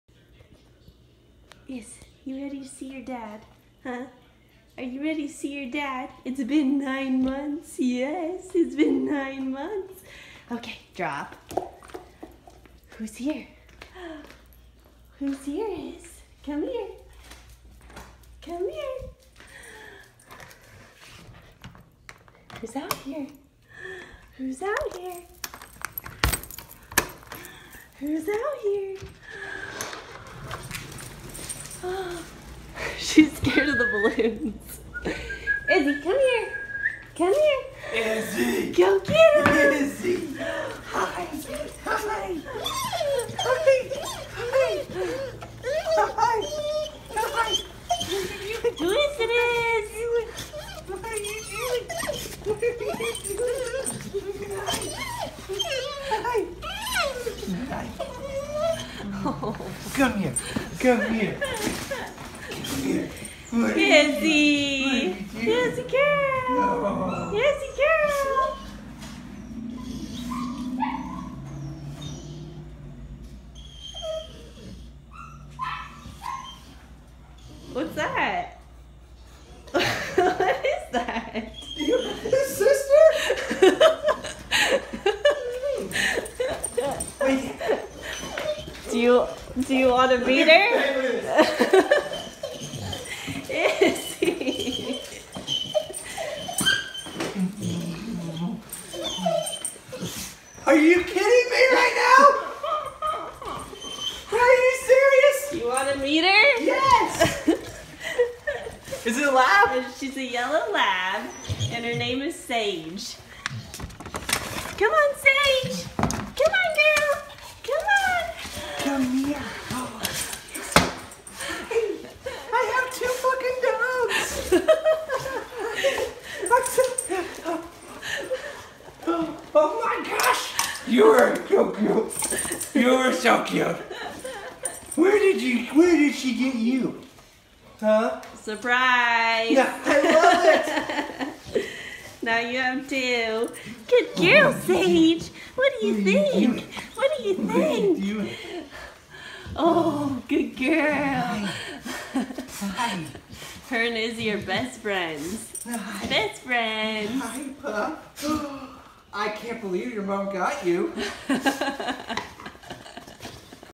Soldier Surprises Dog & Gets Surprised With New Puppy
Our 4.5 year old golden retriever, Izzy, cries when she sees her Dad for the first time after his third deployment. Then he gets surprised with our new yellow lab puppy, Sage!